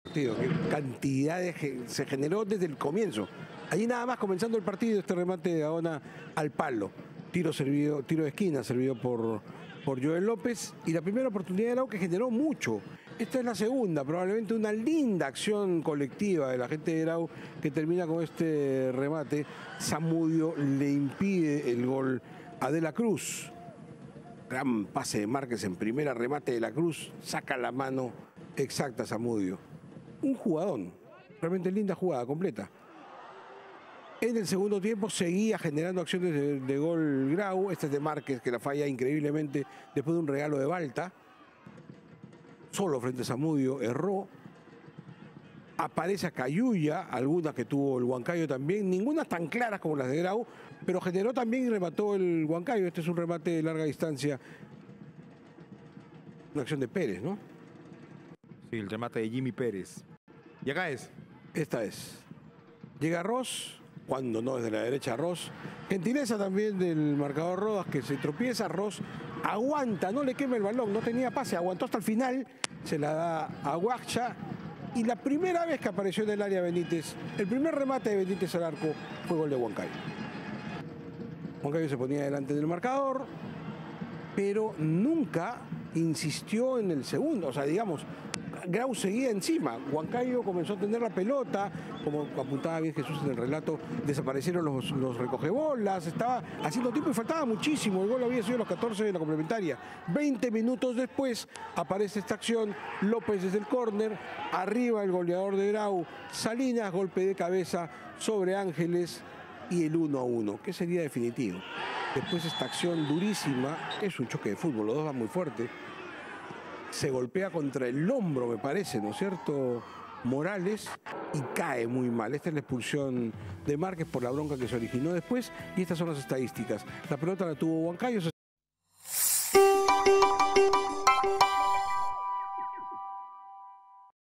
Resumen: Sport Huancayo vs Atlético Grau (1-1)
Liga1 Betsson 2022
Torneo Clausura - Fecha 2
Gol: Luis Benites (SHU) / Rodrigo Salinas (GRA)